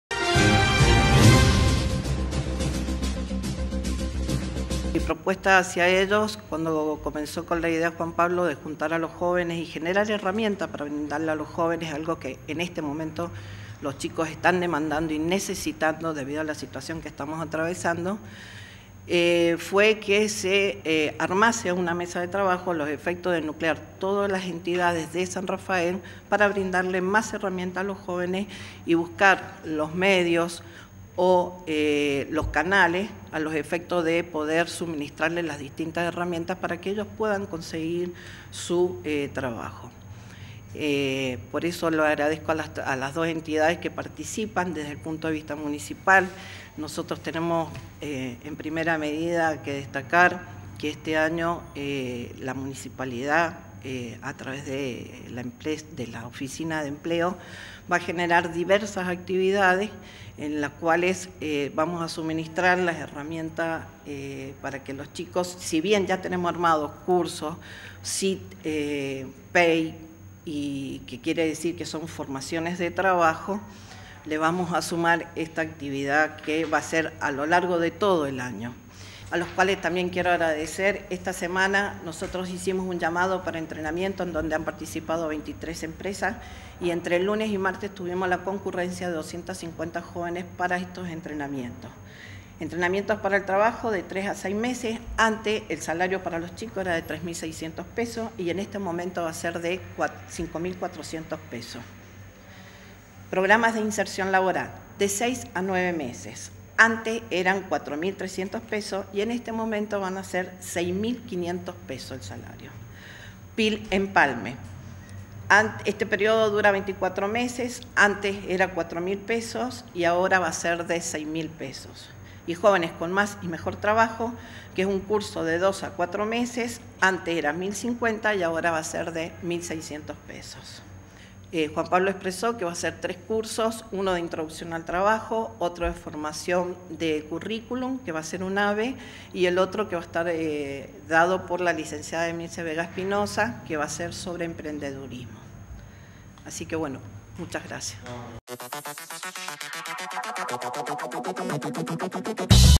GENERANDO HERRAMIENTAS
Fabiana Esperanza, responsable del Área de Empleo de la Municipalidad de San Rafael comentó de qué manera se realizó esta integración entre entidades para brindar herramientas a los jóvenes que buscan trabajo.